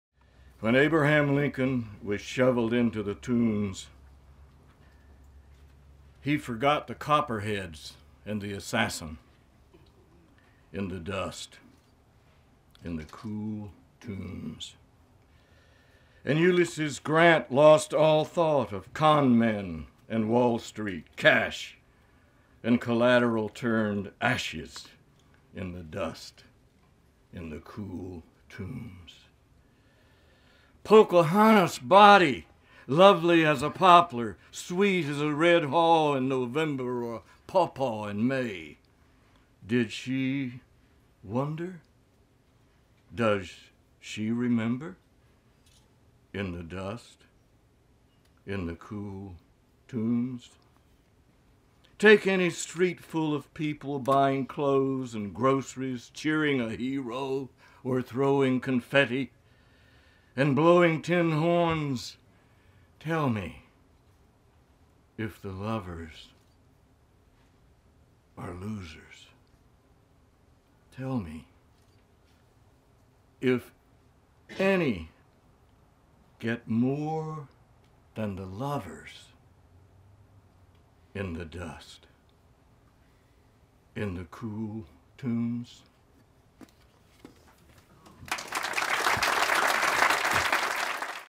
Shelby Stephenson Reads Carl Sandburg
North Carolina Poet Laureate Shelby Stephenson reads Carl Sandburg's poem "Cool Tombs" as part of Carl's induction into the North Carolina Literary Hall of Fame.

Sunday, October 16, 2016, 2:00 pm, at the Weymouth Center for the Arts & Humanities in Southern Pines